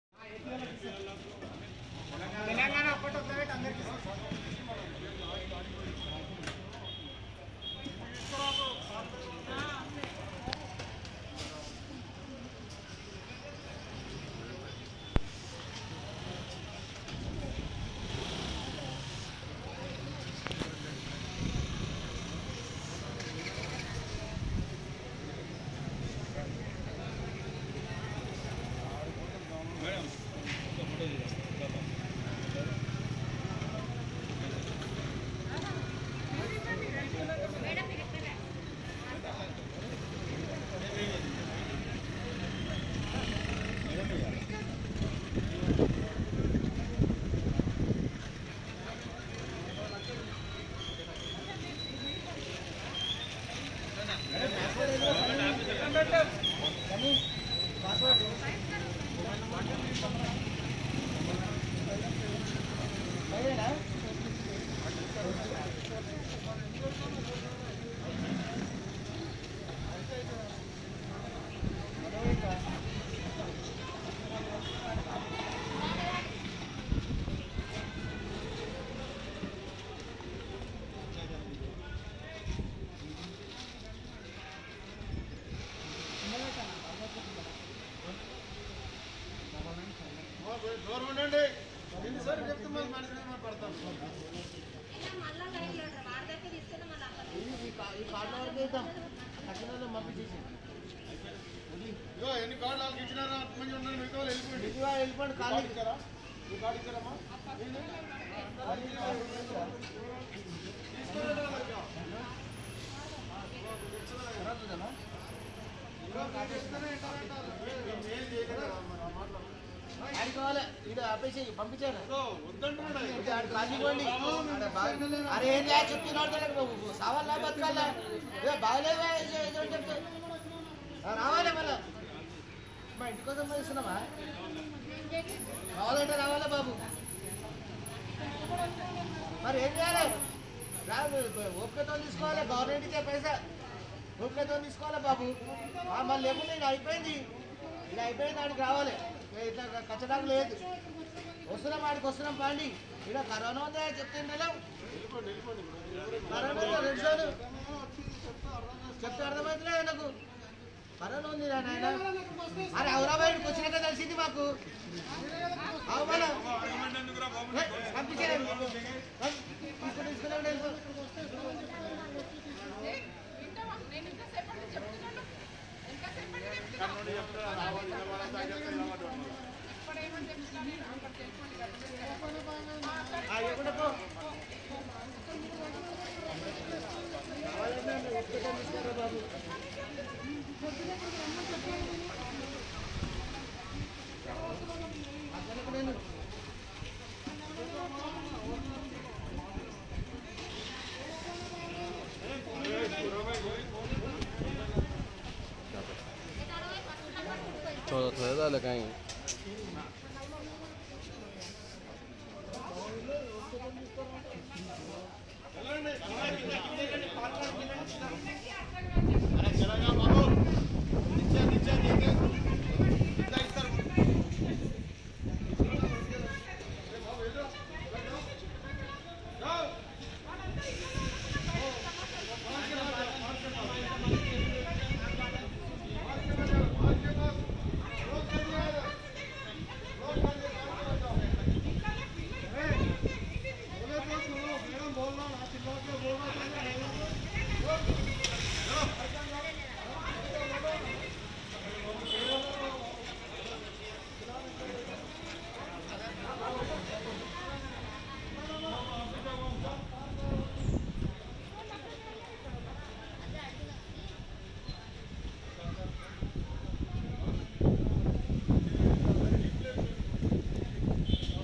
హైదరాబాద్ లో ఉన్న వలస కూలీలకు బియ్యం 12 కిలోలు, రూ.500/- పంచుతున్న.. కేసిఅర్ ప్రభూత్వం
హైదరాబాద్ లో ఉన్న వలస కూలీలకు బియ్యం 12 కిలోలు, రూ.500/- పంచుతున్న..
 కేసిఅర్ ప్రభూత్వం